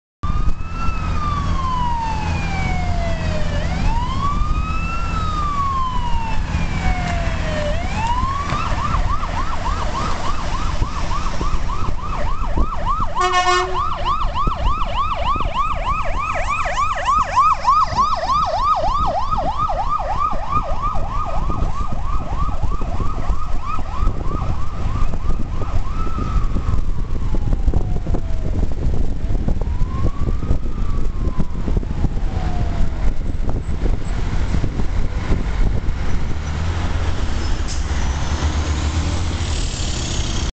Mayfield Village Squad 642
Mayfield Village Squad 642 enroute to Hillcrest.